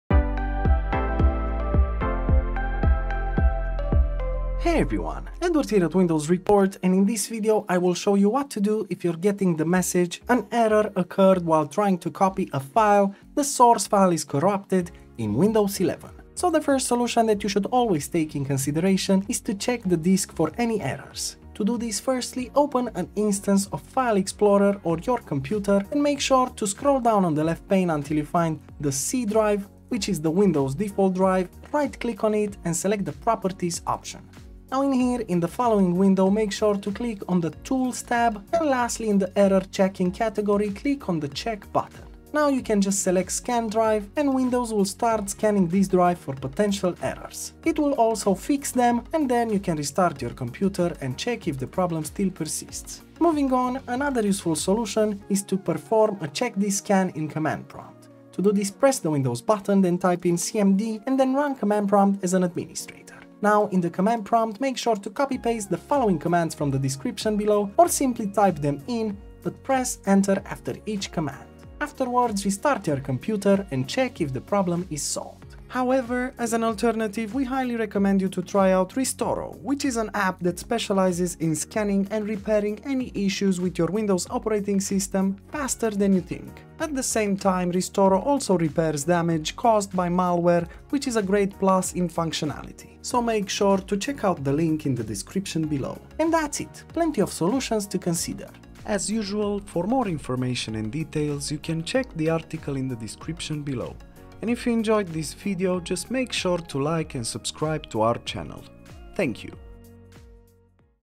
FIX: "An error occurred while trying to copy a file. The source file is corrupted" in Windows 11
| ⚙️Try Fortect to fix and optimize your PC like a Pro

Hello everyone, in this video we will show you how to FIX: "An error occurred while trying to copy a file. The source file is corrupted" in Windows 11.
This error can be troublesome when trying to copy files, but we can take a look at scanning the hard disk for any possible errors.

Commandlines:

1. chkdsk /f
2. chkdsk /r

0:00 - Intro
0:16 - Check the Disk for any Errors
0:53 - Perform a CHKDSK Scan